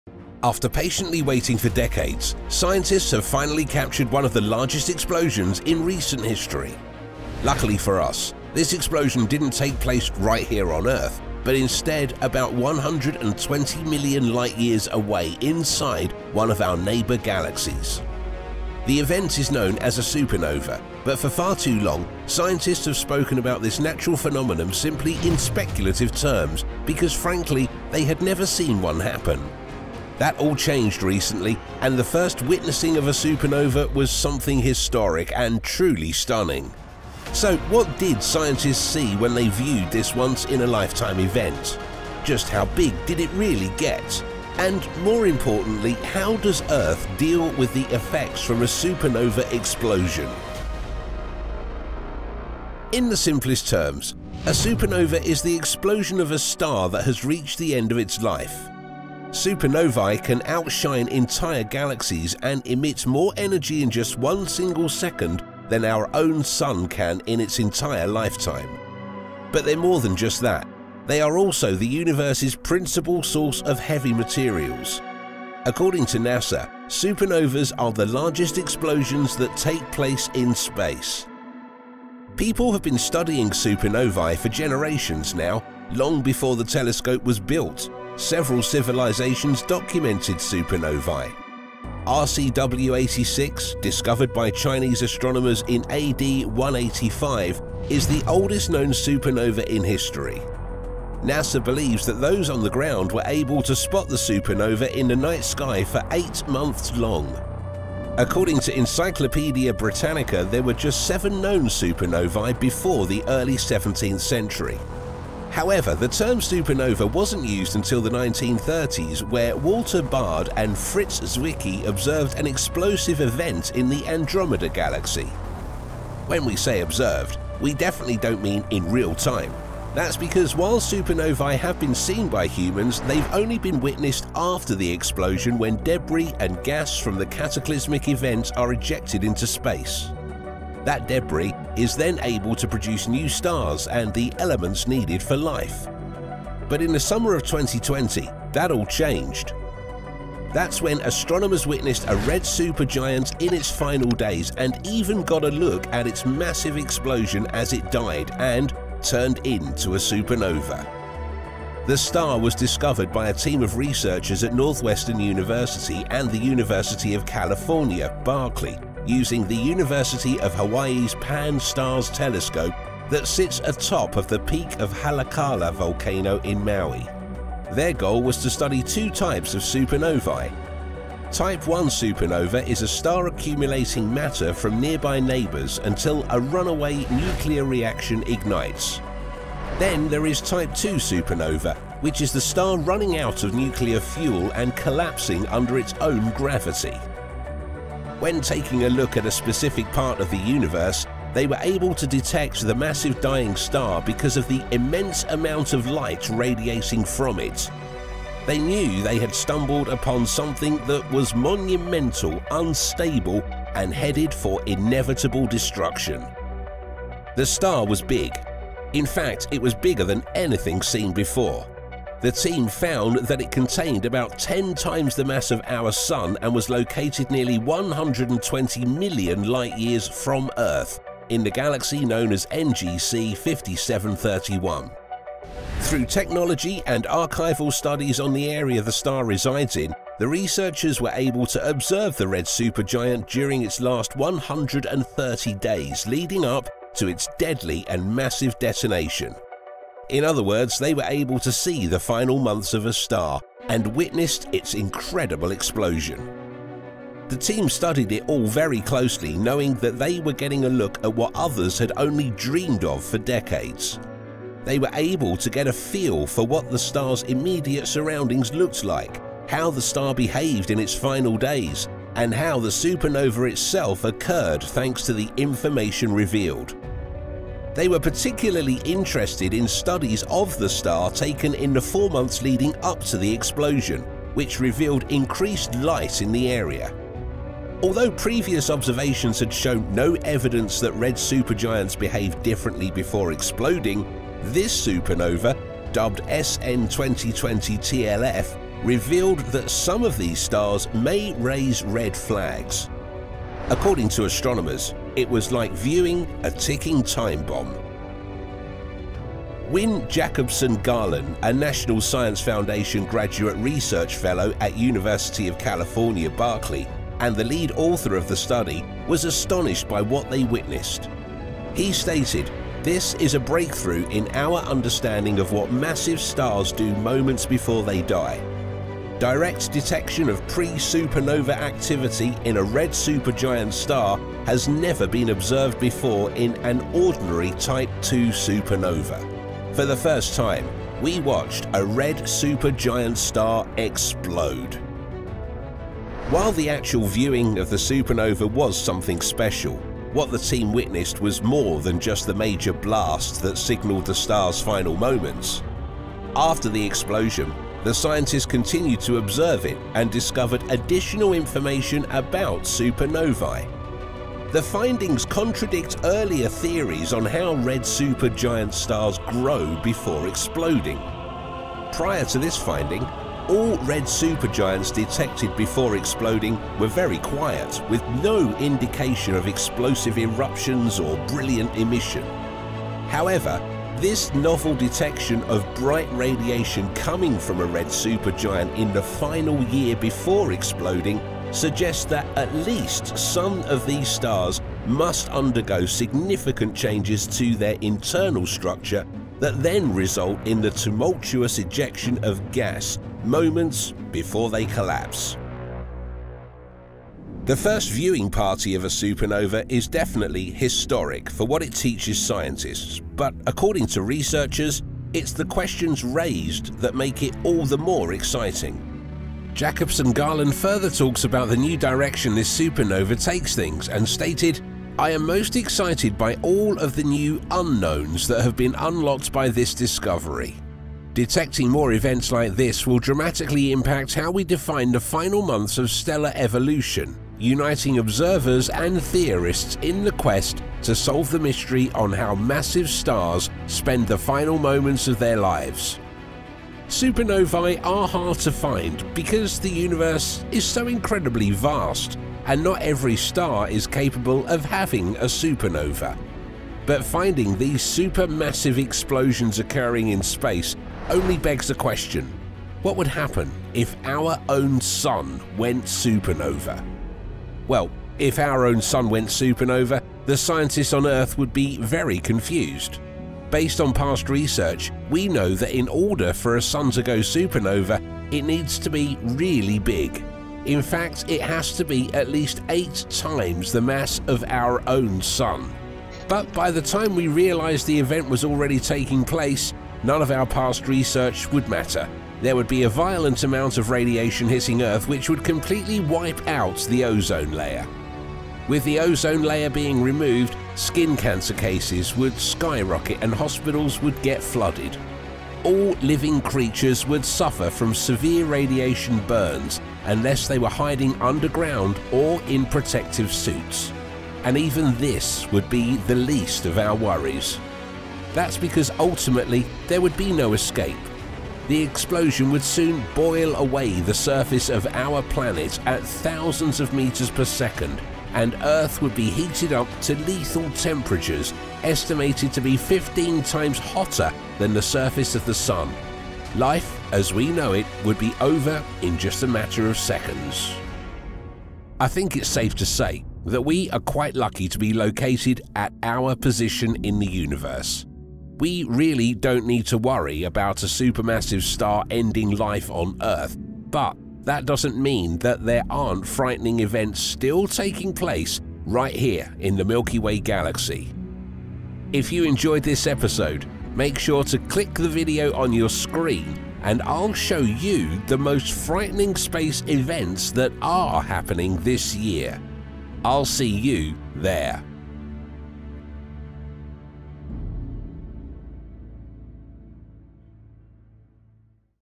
You Will See This Terrifying Supernova Explosion In The Sky!
After patiently waiting for decades, scientists have finally captured one of the largest explosions in recent history. Luckily for us, this explosion didn't take place right here on Earth, but instead about 120 million light-years away inside one of our neighbor galaxies. This event is known as a supernova, but for far too long, scientists have spoken about this natural phenomenon simply in speculative terms because frankly, they had never seen one happen. That all changed recently and the first witnessing of a supernova was something historic and truly stunning.